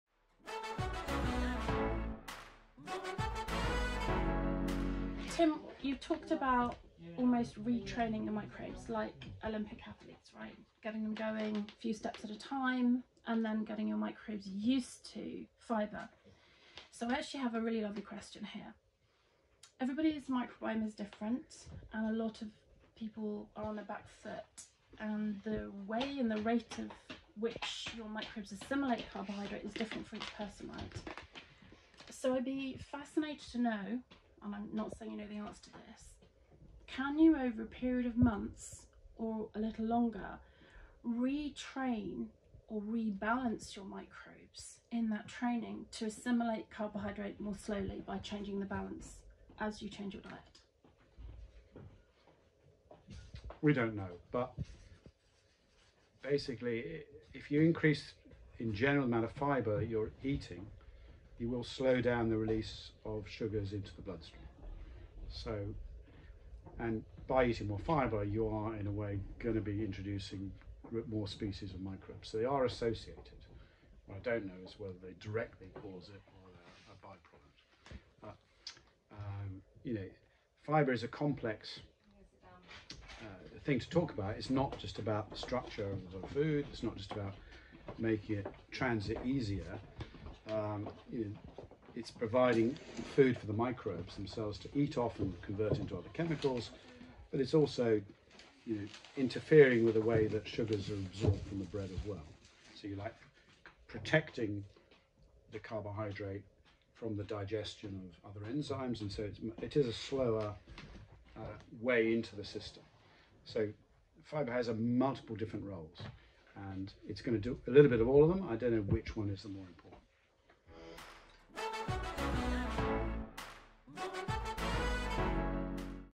Tim Spector Chatting with Vanessa Kimbell about Bread and The Gut Microbiome
Vanessa asks Tim Spector if you can retrain your microbes to assimilate carbohydrate more slowly?